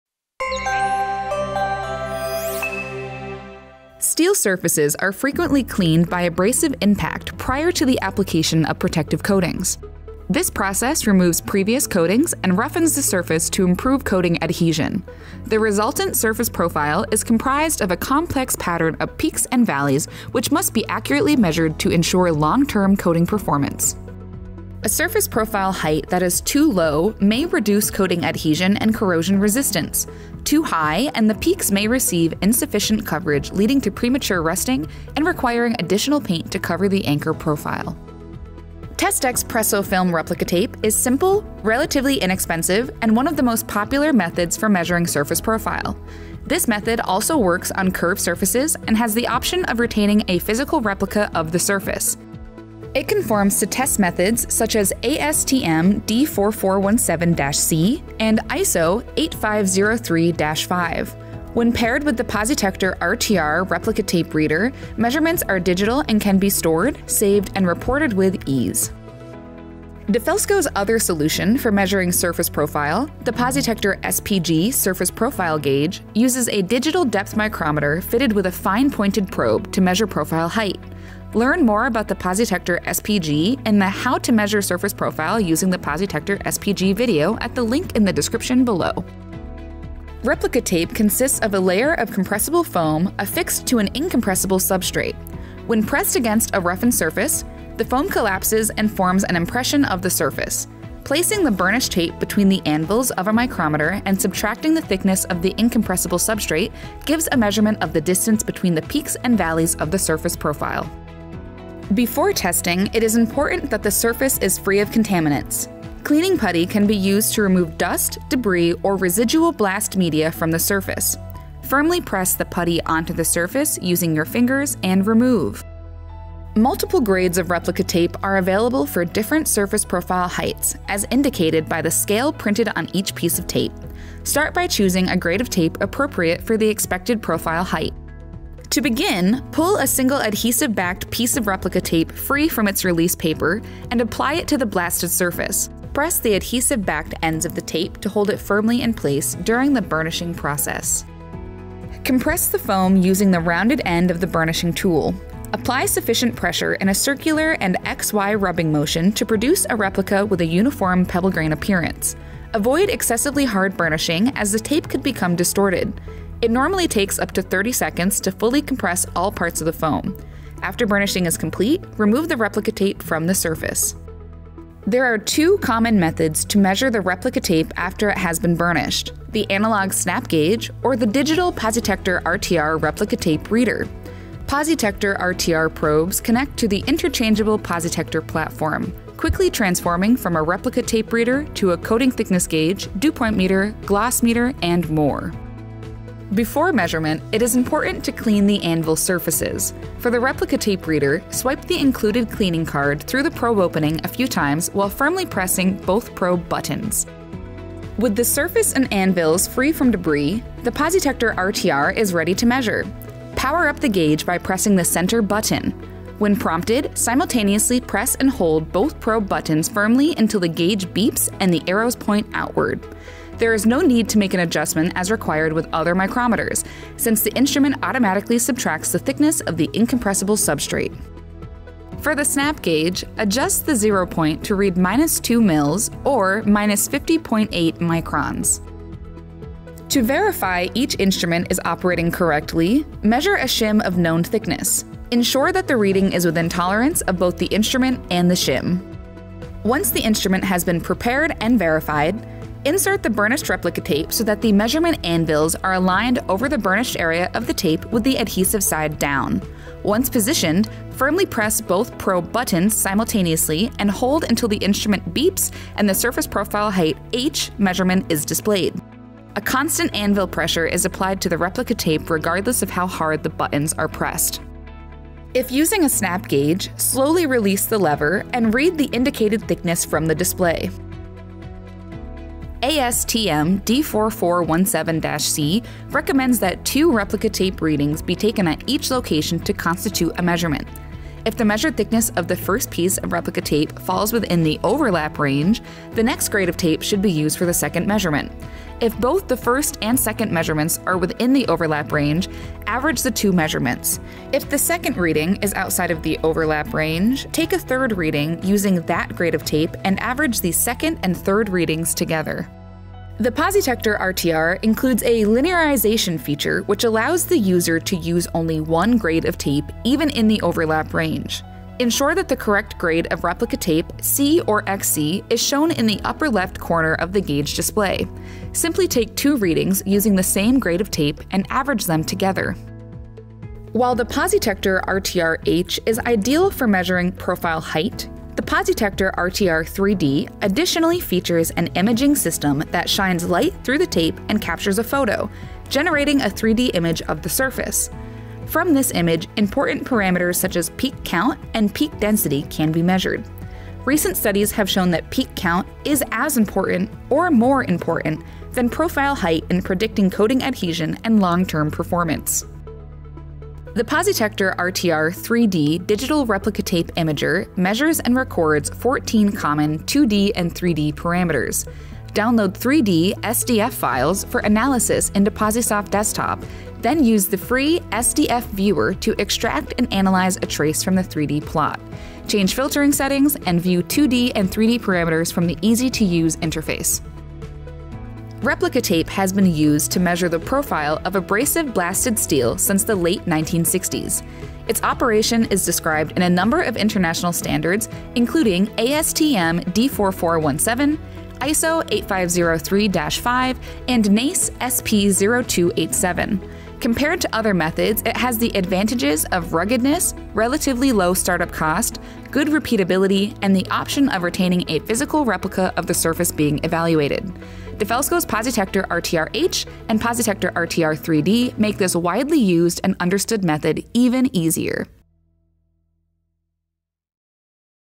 How to Measure Surface Profile Using Testex Replica Tape and the PosiTector RTR
In this video, learn how to measure surface profile using Testex Replica Tape and the PosiTector RTR H and PosiTector RTR 3D Replica Tape Readers.

The PosiTector RTR H and PosiTector RTR 3D are digital spring micrometers that measure and record surface profile parameters using Testex Press-O-Film™ replica tape—producing a more accurate peak-to-valley surface profile height measurement for blasted steel and textured coatings.

PosiTector RTR features:
• Ready to measure—no calibration adjustment required for most applications
• Minimizes inspector workload by reducing the number of replicas needed to ensure accuracy
• PosiTector interchangeable platform—attach any PosiTector probe to a single gage body
• USB, WiFi, and Bluetooth connectivity to PosiSoft PC, Mac, and Smartphone software
• Weatherproof, dustproof, and water-resistant—IP65-rated enclosure
• Standard or Advanced models available

Chapters:
0:00 — Intro—Why measure surface profile?
0:39 — What is Testex Press-O-Film™ Replica Tape and how does it measure surface profile?
1:55 — How to measure surface profile using replica tape
5:12 — ASTM D4417—Replica tape linearization with and without the PosiTector RTR H
5:33 — How to measure 3D surface profile parameters with the PosiTector RTR 3D
6:28 — Outro